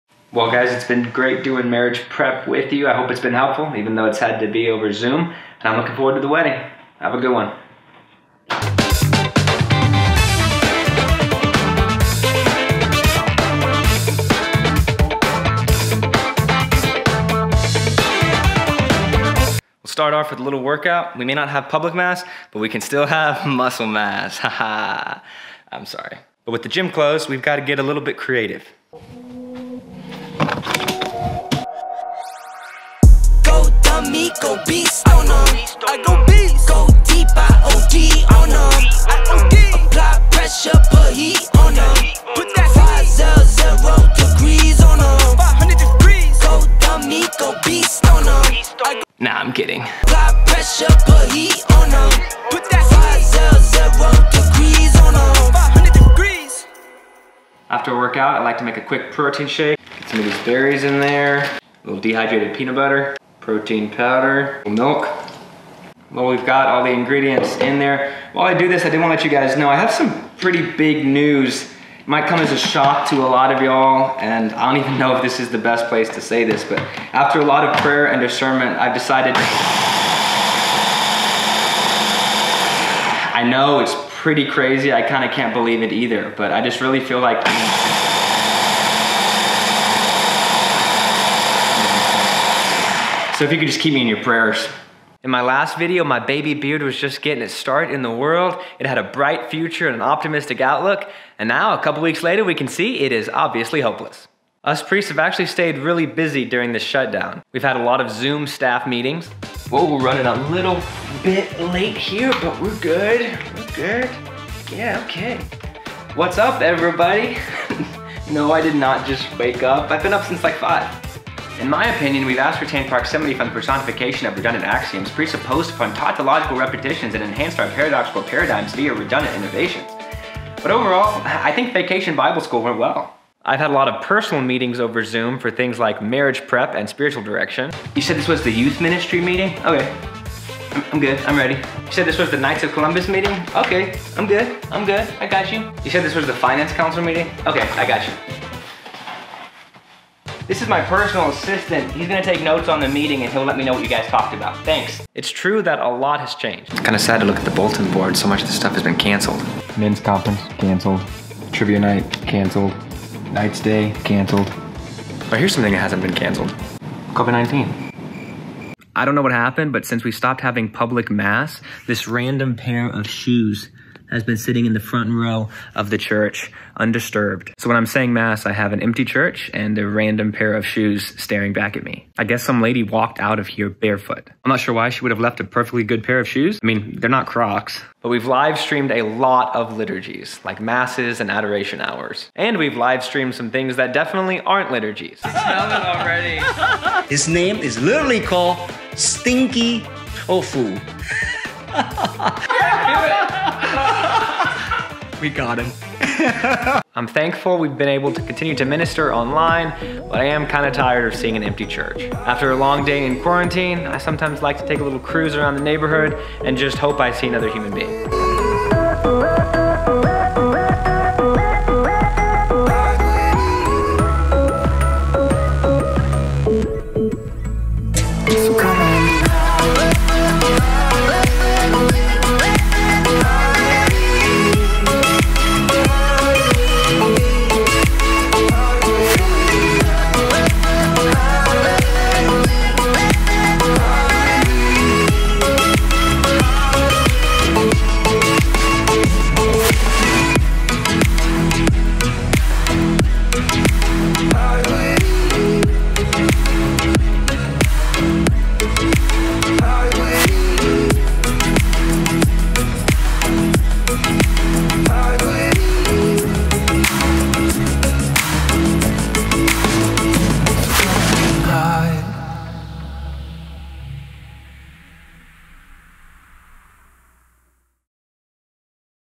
A Catholic Priest in Quarantine
Here's what I've been up to during the coronavirus shutdown.